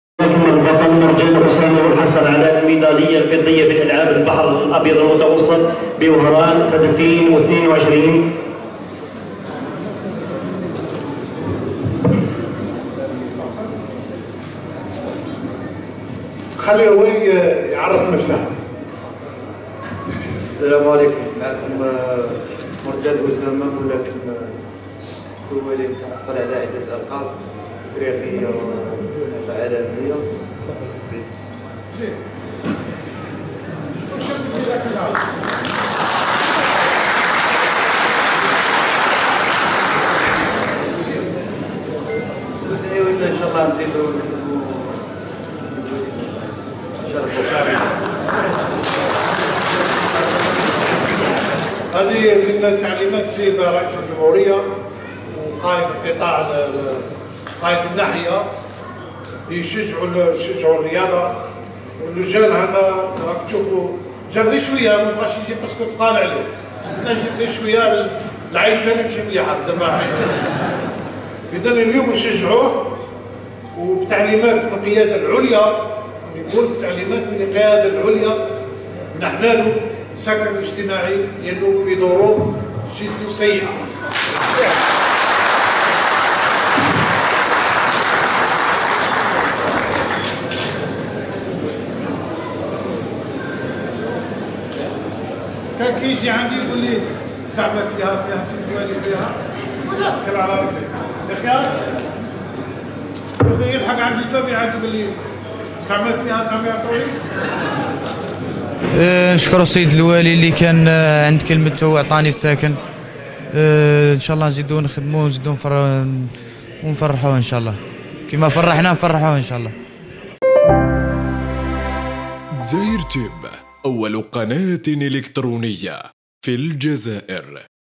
شااااهد.... والي ميلة يمازح الملاكم العالمي أسامة مرجان بالقول... ما جابش كامل الميداليات تاعو
شااااهد. والي ميلة يمازح الملاكم العالمي أسامة مرجان بالقول... ما جابش كامل الميداليات تاعو... خايف من العين...ويمنحه سكن إجتماعي